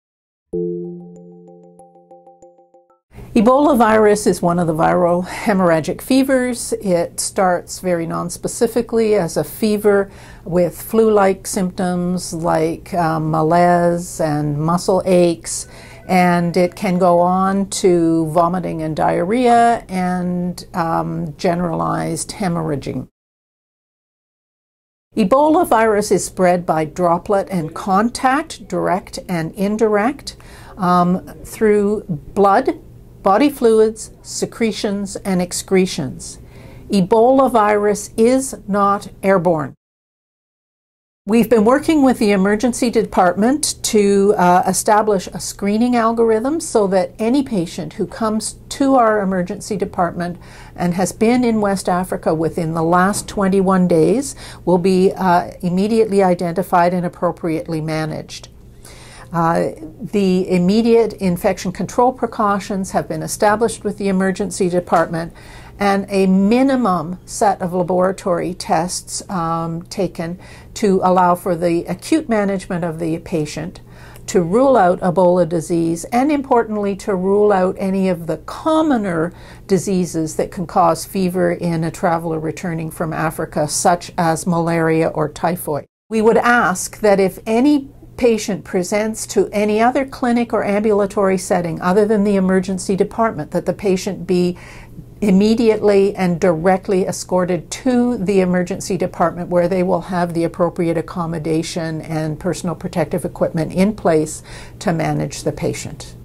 Ebola: Dr. Vearncombe explains surveillance + screening tools
Learn more about our surveillance tools, including our screening algorithm for the ED. Watch this video with Dr. Mary Vearncombe, medical director, Infection Prevention & Control.